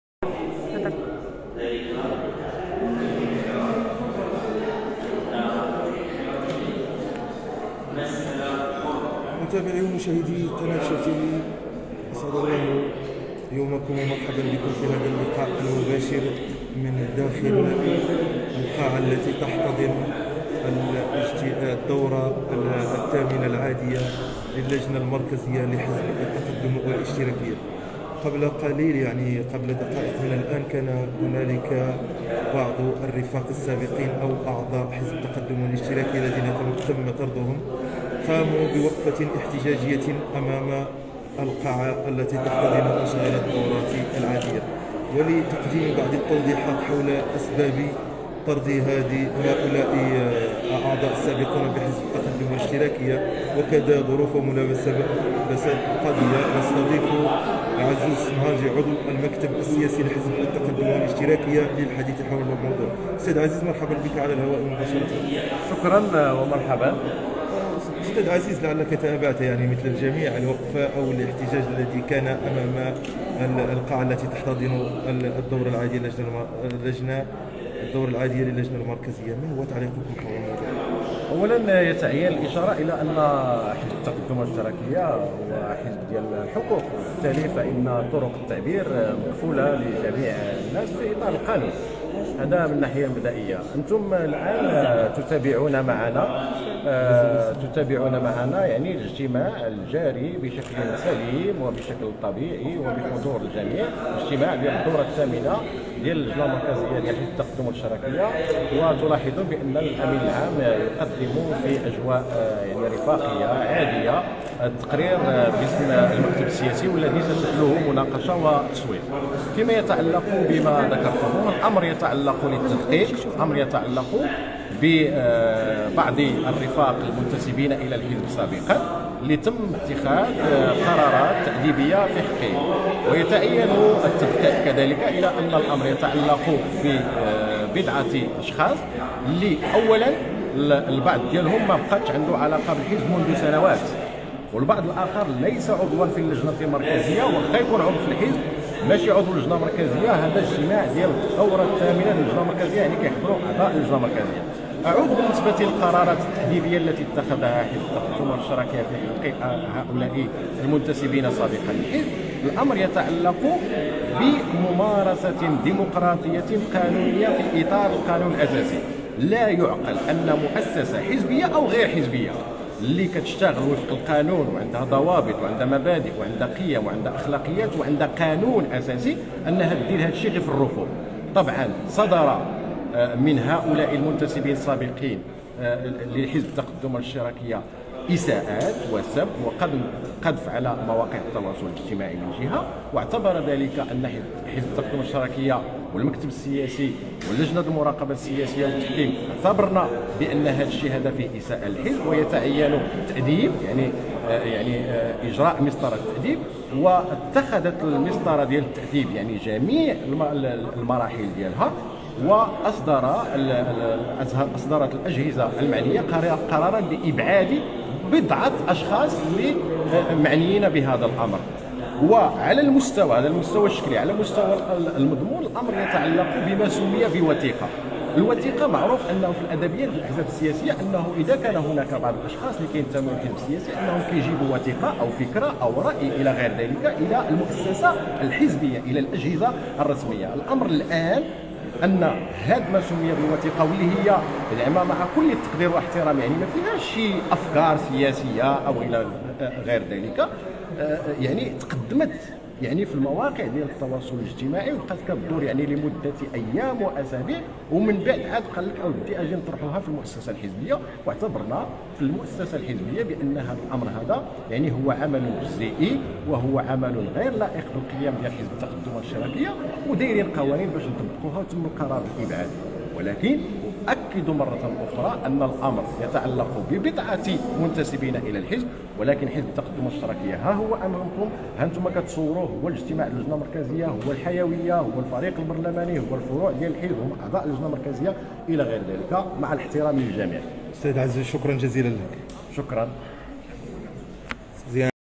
عضو المكتب السياسي لحزب التقدم والاشتراكية يوضح أسباب القربالة وطرد أعضاء من الحزب وها شنو وقع
عاجل وعلى المباشر من الرباط:عضو المكتب السياسي لحزب التقدم والاشتراكية عزوز الصنهاجي يوضح أسباب القربالة وطرد أعضاء من الحزب وها شنو وقع